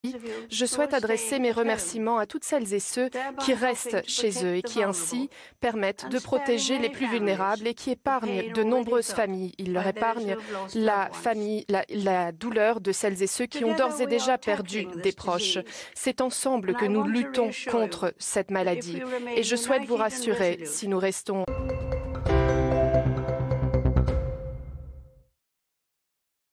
Elizabeth II : « Si nous restons déterminés, nous serons en mesure de surmonter cette crise »
La Reine d'Angleterre, Elizabeth II, s'est exprimée ce dimanche soir à propos de la crise actuelle du coronavirus qui touche depuis quelques semaines déjà le Royaume-Uni et plus globalement le monde entier désormais.